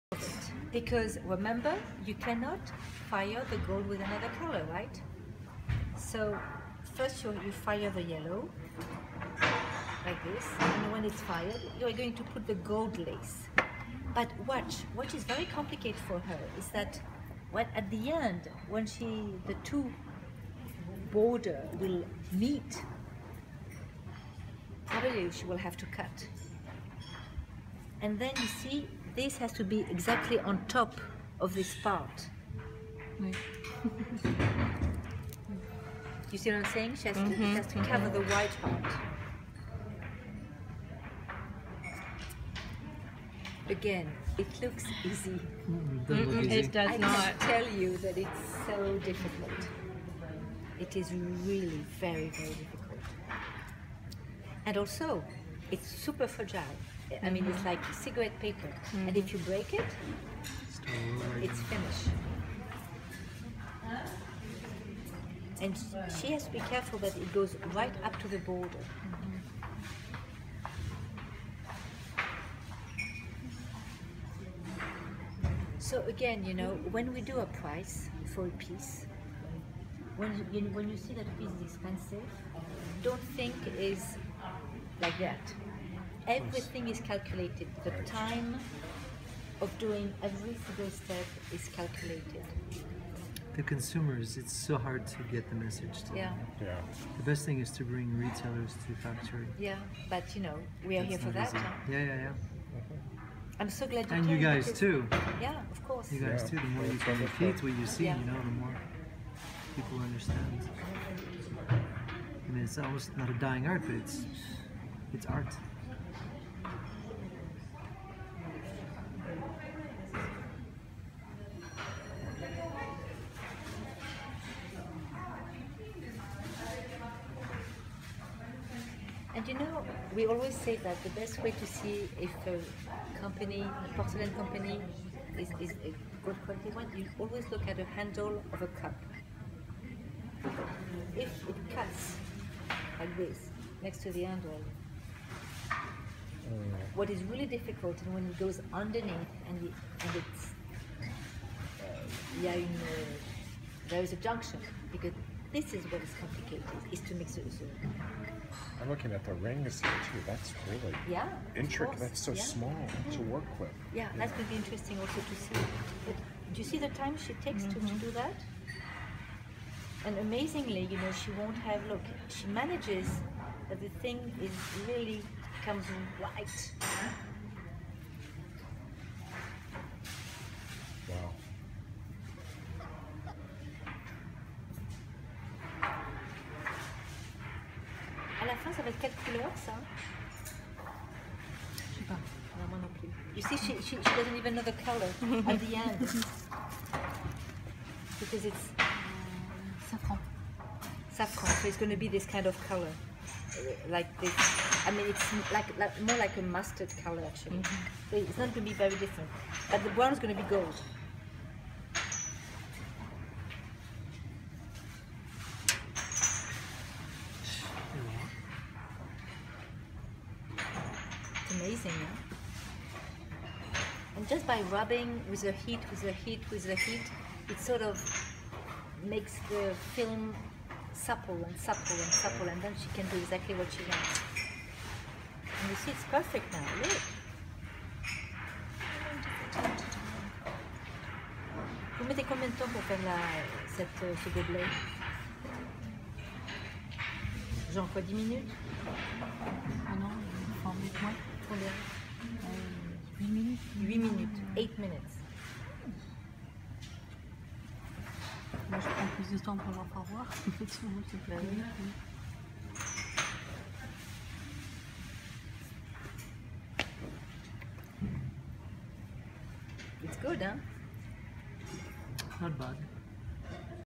Decorating Porcelain at Bernardaud
One of Bernardaud's skilled porcelain artisans wraps a tumbler in a decorative pattern.